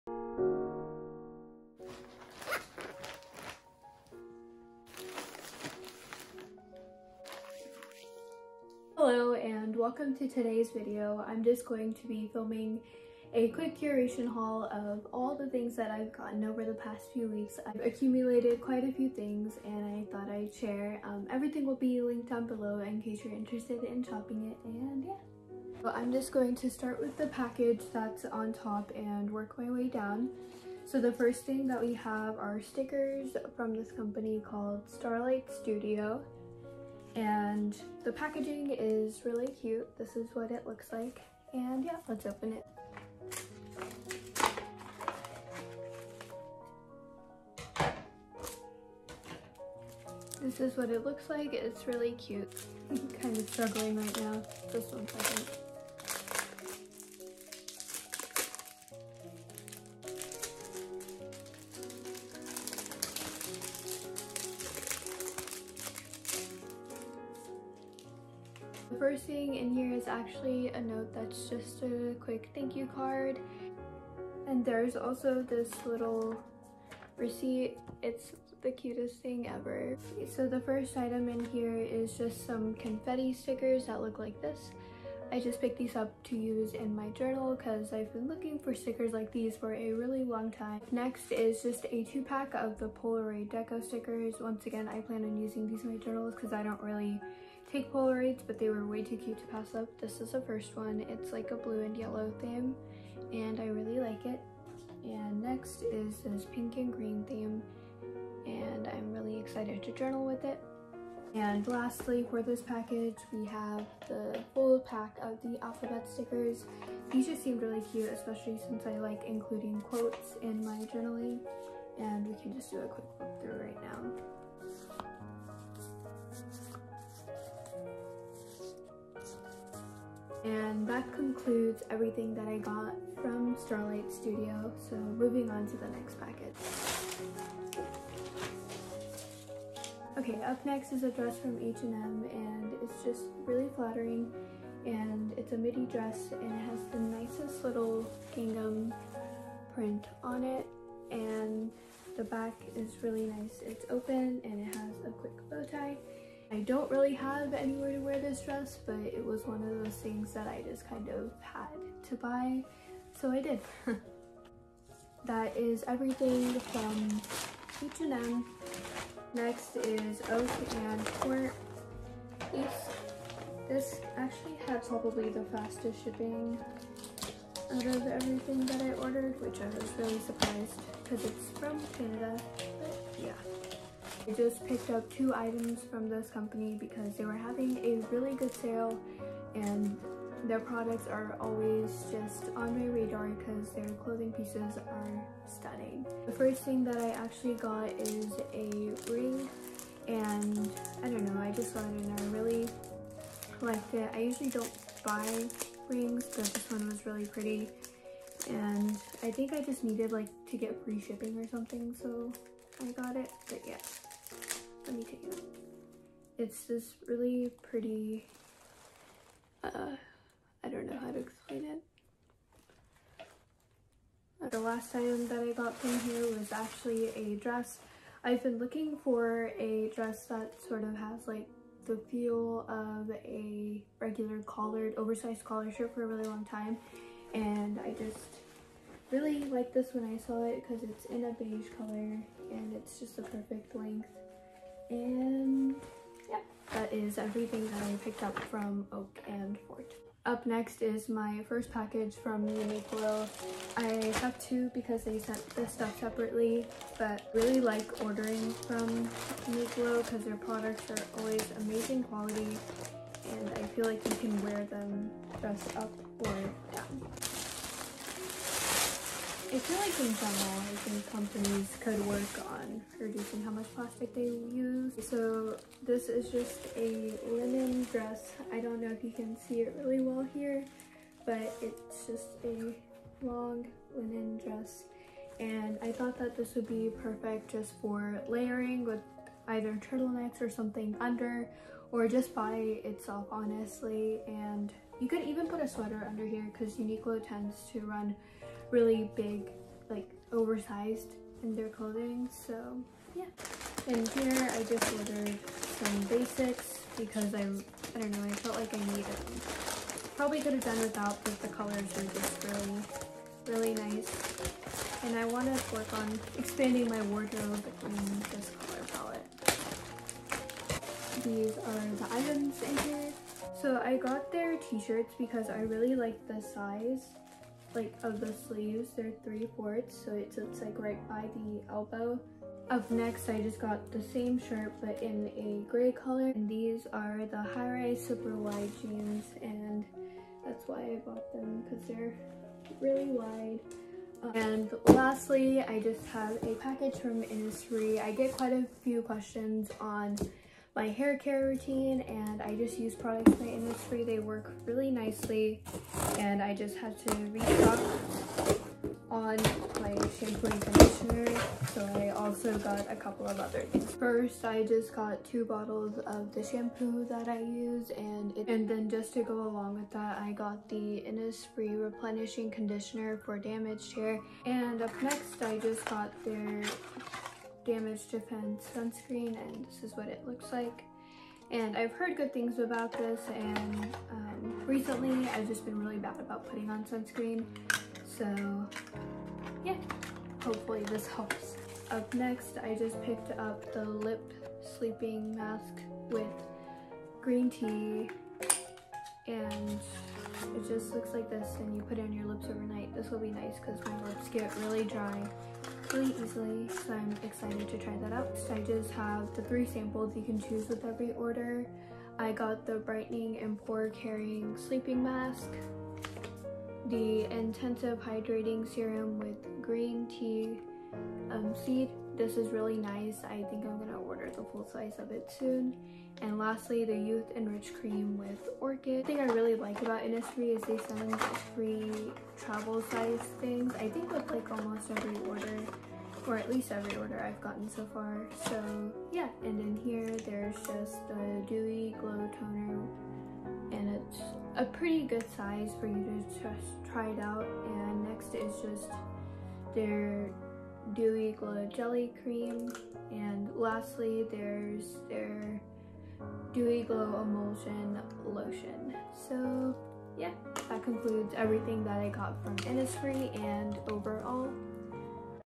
curation haul no. 1
Hello!

Today's video is a haul of all the things I’ve bought over the past few weeks.

uka
// links
(stxrlight studio’s website is down)

beige dress: out of stock

// amazon wishlist

// profile picture by @chow.lien on instagram